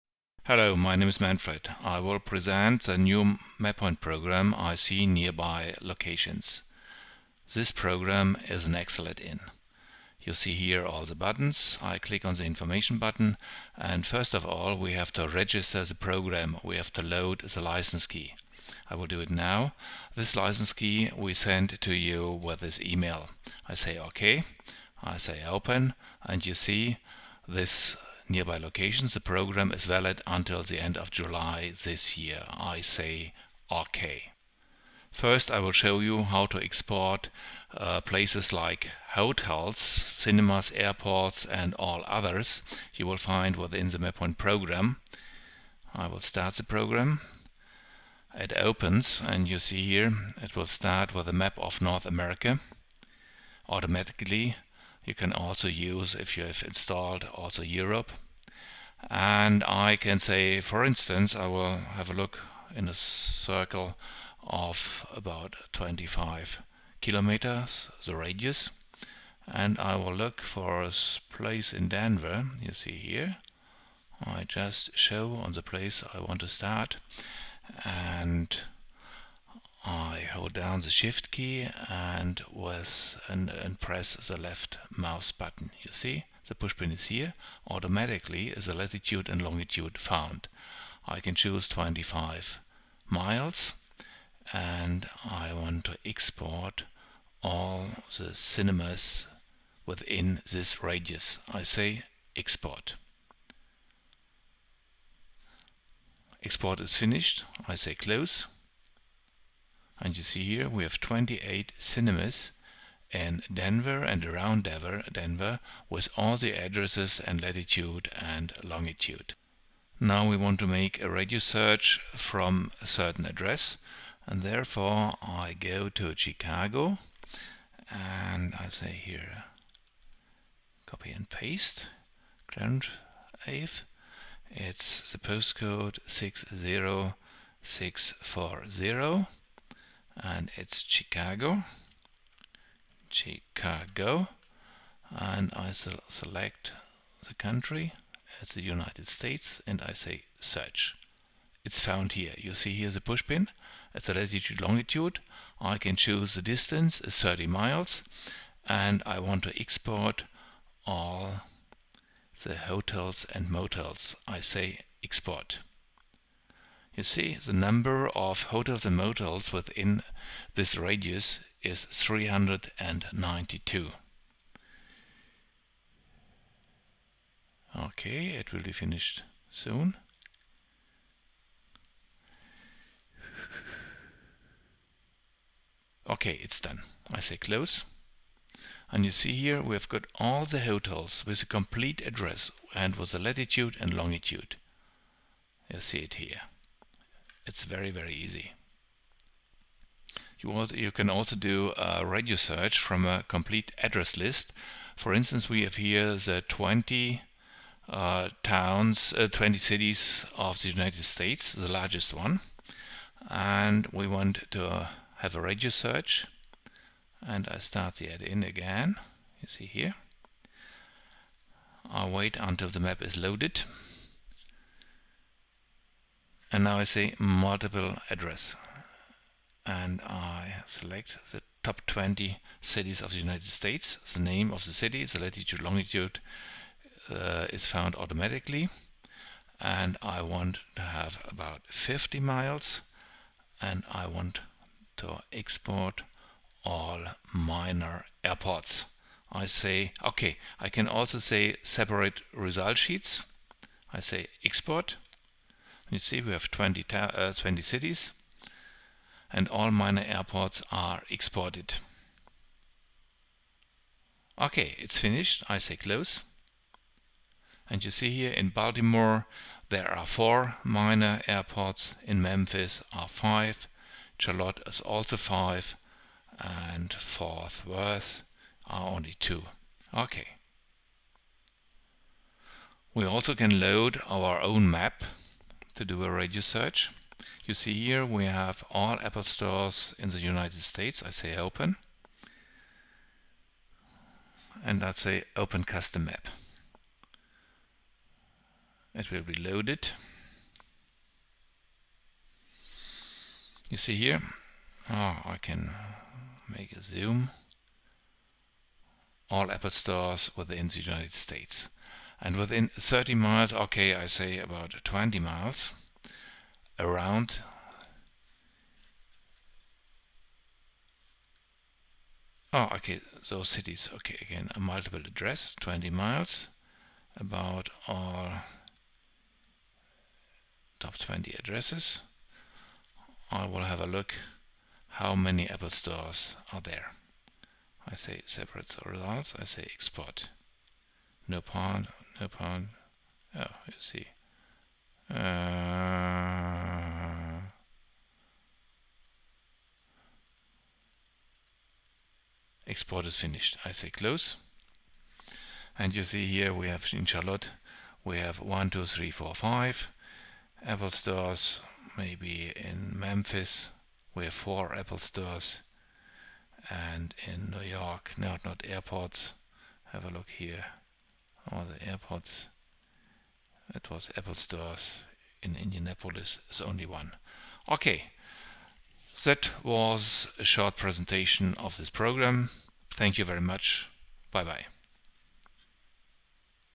Microsoft MapPoint® : New Excel Addin - radius search
We invite you, to test our newest MapPoint Excel-AddIn:  "IC-NearbyLocations".
The program provides many powerful tools for radius search.